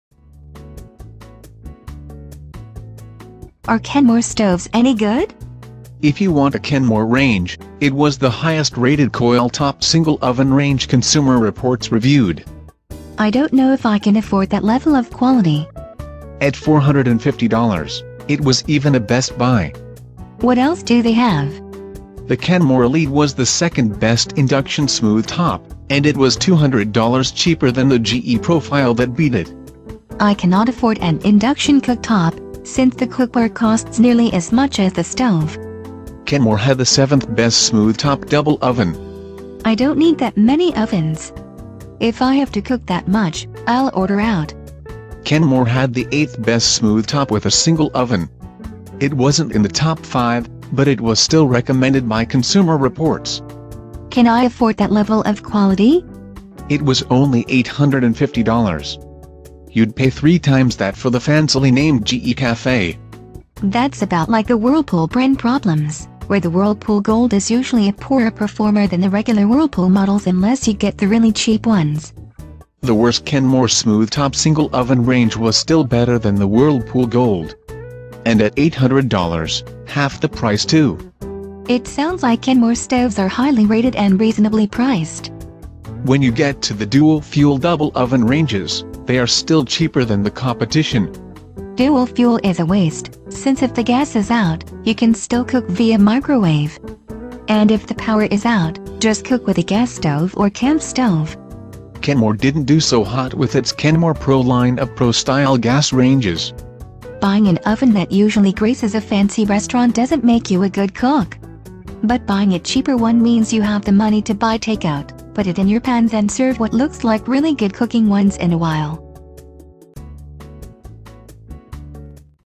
Are Kenmore Stoves Any Good?
If you want a Kenmore range, it was the highest rated coil top single oven range Consumer Reports reviewed.

I don't know if I can afford that level of quality.

At four hundred and fifty dollars, it was even a Best Buy.

What else do they have?

The Kenmore Elite was the second best induction smooth top, and it was two hundred dollars cheaper than the GE Profile that beat it.

I cannot afford an induction cook top, since the cookware costs nearly as much as the stove.

Kenmore had the seventh best smooth top double oven.

I don't need that many ovens. If I have to cook that much, I'll order out.

Kenmore had the eighth best smooth top with a single oven. It wasn't in the top five, but it was still recommended by Consumer Reports.

Can I afford that level of quality?

It was only eight hundred and fifty dollars. You'd pay three times that for the fancily named GE Cafe.

That's about like the Whirlpool brand problems, where the Whirlpool Gold is usually a poorer performer than the regular Whirlpool models unless you get the really cheap ones.

The worst Kenmore smooth top single oven range was still better than the Whirlpool Gold. And at eight hundred dollars, half the price, too.

It sounds like Kenmore stoves are highly rated and reasonably priced.

When you get to the dual fuel double oven ranges, they are still cheaper than the competition.

Dual fuel is a waste, since if the gas is out, you can still cook via microwave. And if the power is out, just cook with a gas stove or camp stove.

Kenmore didn't do so hot with its Kenmore Pro line of Pro-style gas ranges.

Buying an oven that usually graces a fancy restaurant doesn't make you a good cook. But buying a cheaper one means you have the money to buy takeout, put it in your pans and serve what looks like really good cooking once in a while.